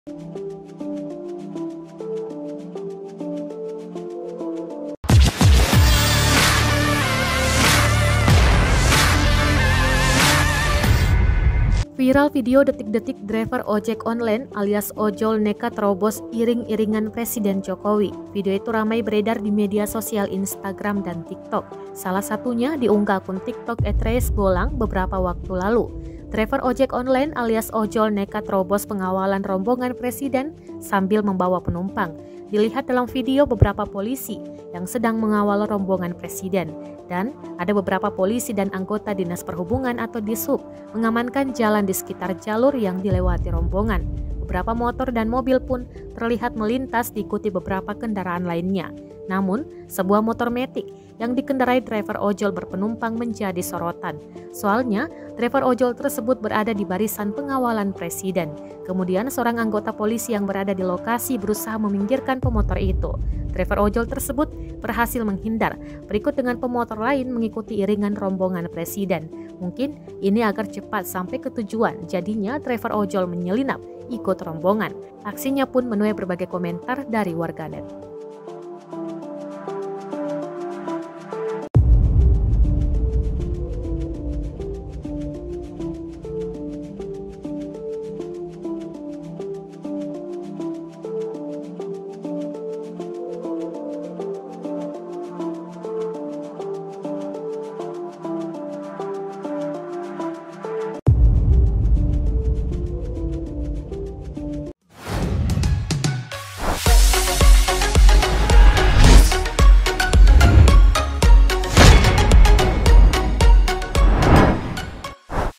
VIRAL Detik detik Driver Ojol Berpenumpang Nekat Terobos Iring iringan Presiden
TRIBUN-TIMUR.COM - Viral video detik-detik driver ojol nekat terobos iring-iringan Presiden Jokowi.

Salah satunya diunggah akun TikTok @reisebolang beberapa waktu lalu.

Driver ojek online alias ojol nekat terobos pengawalan rombongan presiden sambil bawa penumpang.

Dilihat dalam video beberapa polisi yang sedang mengawal rombongan Presiden.

Dan ada beberapa polisi dan anggota Dinas Perhubungan (Dishub) mengamankan jalan di sekitar jalur yang dilewati rombongan.

Beberapa motor dan mobil pun terlihat melintas diikuti beberapa kendaraan lainnya.

Namun, sebuah motor matic yang dikendarai driver ojol berpenumpang menjadi sorotan.

Soalnya, driver ojol tersebut berada di barisan pengawalan presiden.

Kemudian, seorang anggota polisi yang berada di lokasi berusaha meminggirkan pemotor itu.

Driver ojol tersebut berhasil menghindar berikut dengan pemotor lain mengikuti iringan rombongan presiden.

Mungkin ingin cepat sampai ke tujuan jadinya driver ojol menyelinap ikuti rombongan.

Aksinya pun menuai berbagai komentar dari warganet.

Narator video: Nur Fajriani

(TRIBUN-TIMUR.COM)